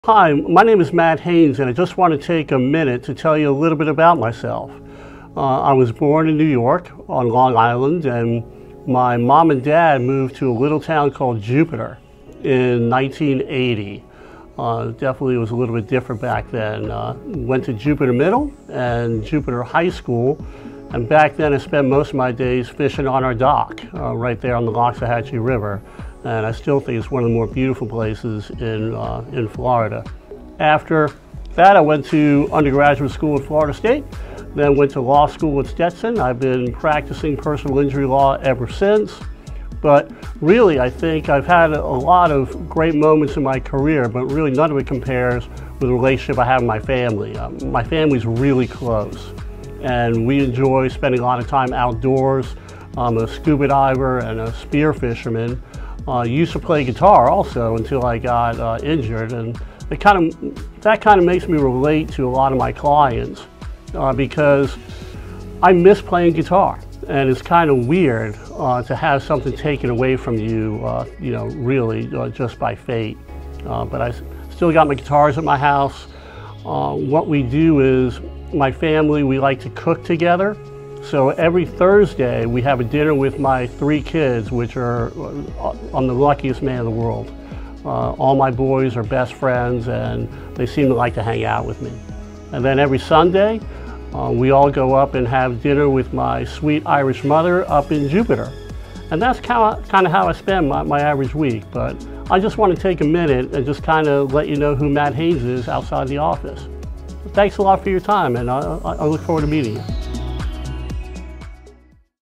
Matthew Haynes | About Me | For Your Rights
Matthew Haynes has a state-wide reputation as a successful trial lawyer. Mr. Haynes has tried numerous cases involving wrongful death; automobile negligence; premises liability, negligent security; uninsured motorist coverage; employment discrimination; civil RICO violations; First Amendment violations; “whistle-blower” violations; and, breaches of fiduciary duty, cases to verdict. Before joining Lytal, Reiter, Smith, Ivey, and Fronrath, he was frequently retained as co-counsel by firms throughout Florida to assist them in presenting cases to juries.